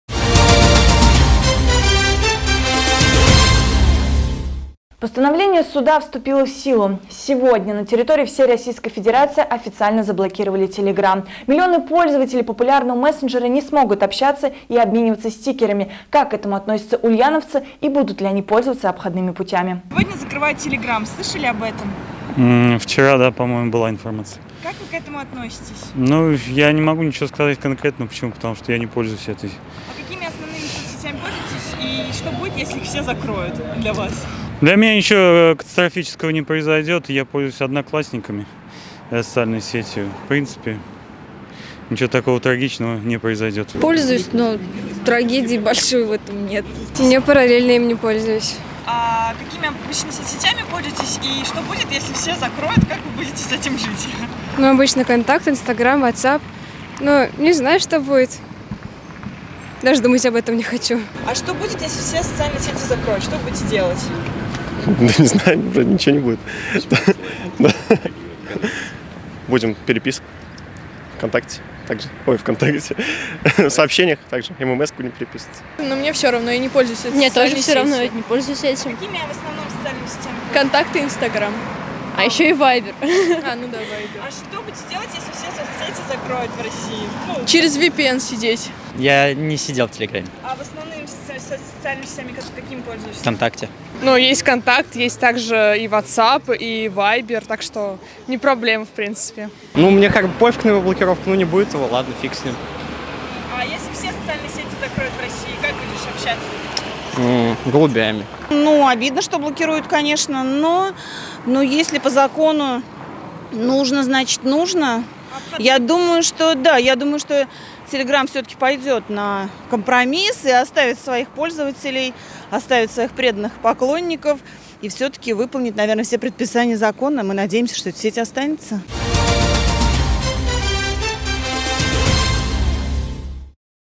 Посылать голубей и MMC. Что будут делать ульяновцы без популярных соцсетей
13 апреля московский суд принял решение о блокировке Telegram на территории России. Сегодня постановление вступает в законную силу. Месенджер официально перестанет функционировать в России. Как к этому относятся жители Ульяновска и будут ли пользоваться обходными путями?